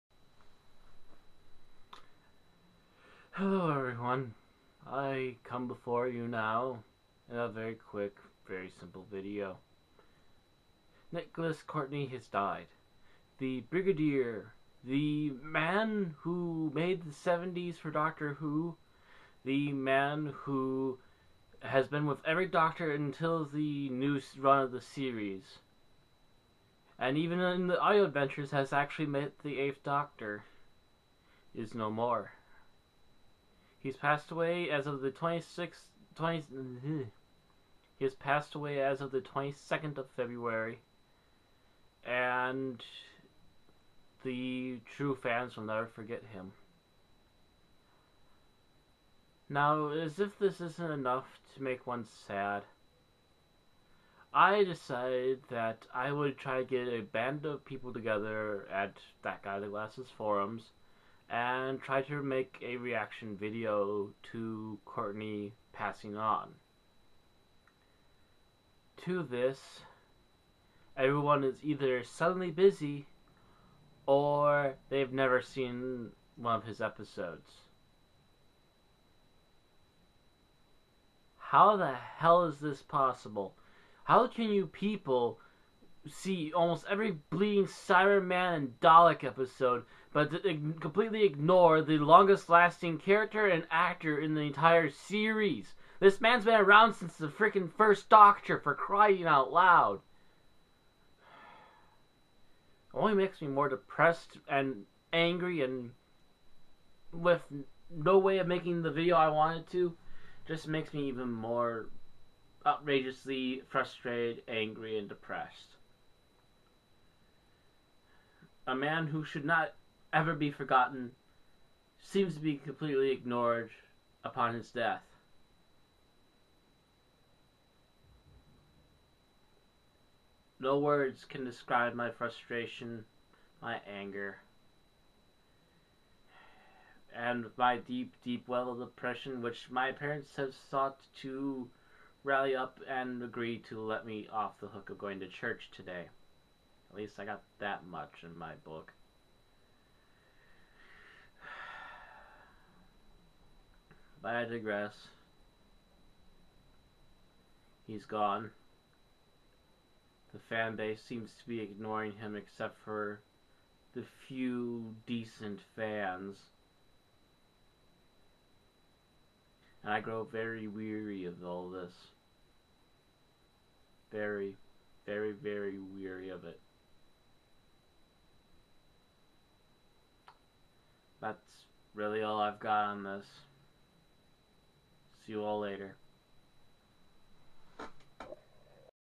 Matt V-log 1: Nicholas Courtney is gone
Nicholas Courtney is gone, Brigadier Alistair Gordon Lethbridge-Stewart is no more. And non of the fans have seen his stuff, or are "to busy" for a 30 second reaction clip. My depression and rage know no bounds.